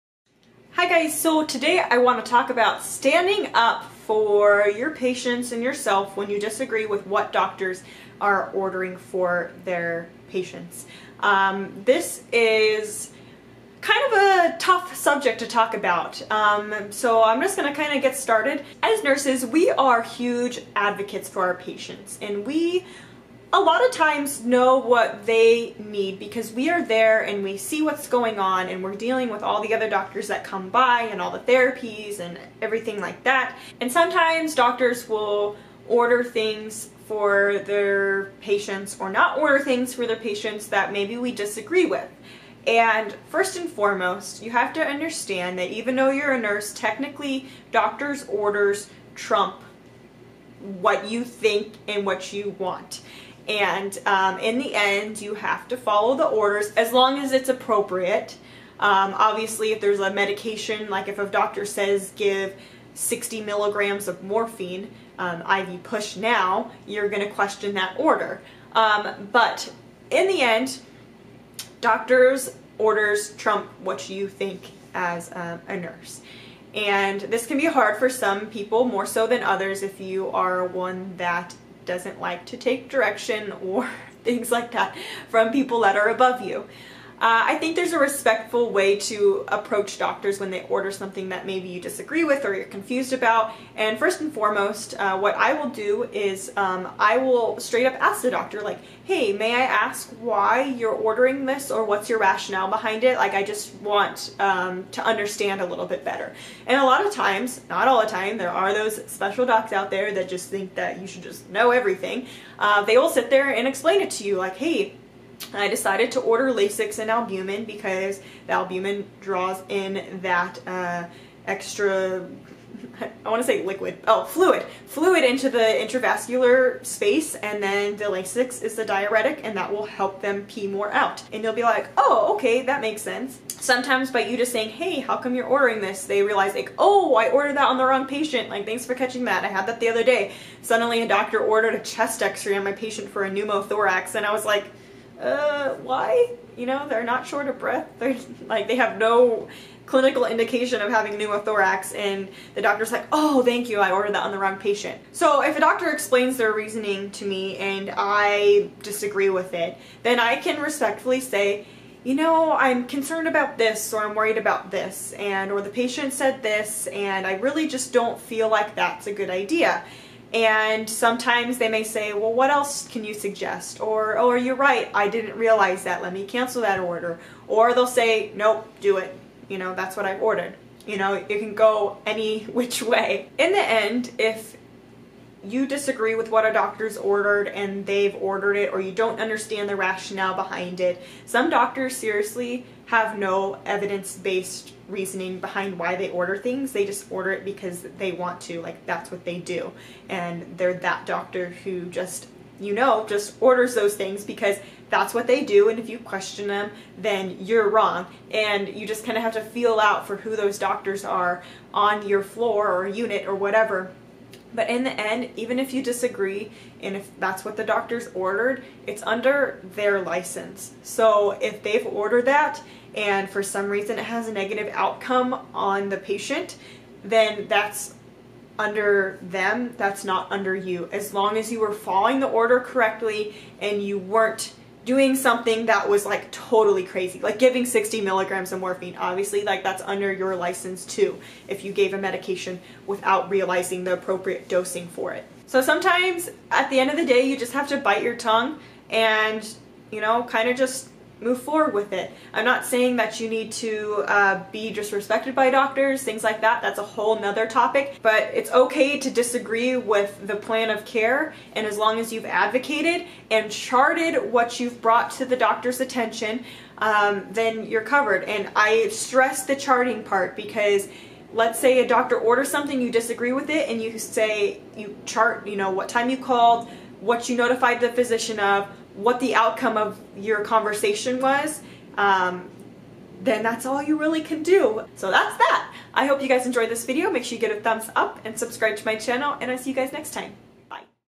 TELLING A DR. YOU DISAGREE WITH THEM!
As nurses, we don't always agree with doctors. Nurses are HUGE patient advocates and it can be difficult when what you feel is right for the patient conflicts with the doctor's opinions.

❥Social Sites❥